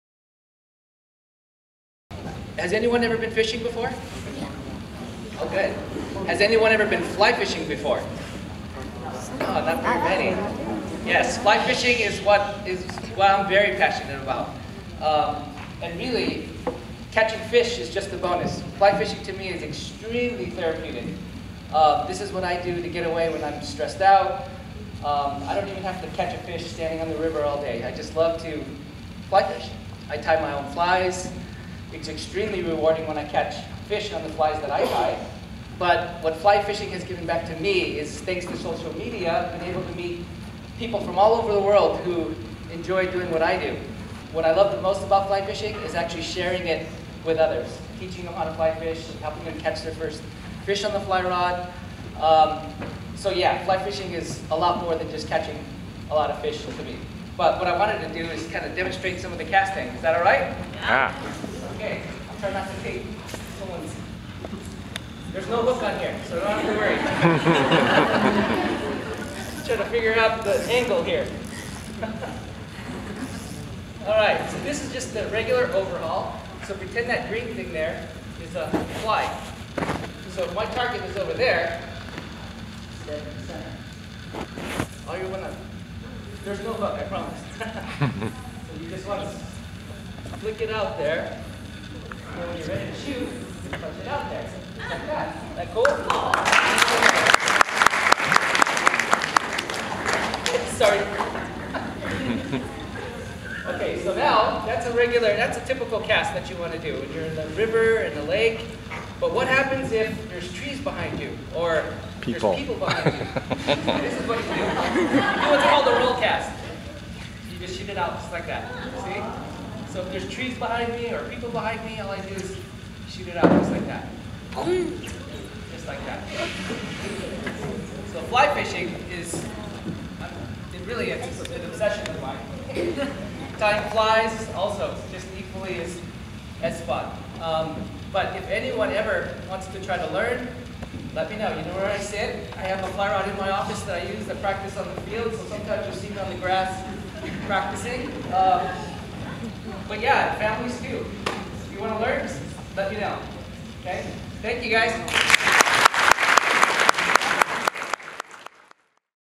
One Minute Passion Talk: Fly fishing by Lino "Nick" Jubilado
Clairbourn's Health Office Attendant Lino “Nick” Jubilado shared his passion for fly fishing during Friday’s One Minute Passion Talk at the Morning Assembly. Outside of school hours, Nick can be found fly-fishing along the L.A. River or giving lectures on fly fishing methods, tips, and techniques at various locations around town. With over thirty years of experience, Nick has become a star within the L.A. River fishing community. In July, Nick was featured in an article by the L.A. Times titled, "Fly fishing for 'sewer salmon' in the L.A. River." Watch Nick’s talk and learn more about why fly fishing is his passion.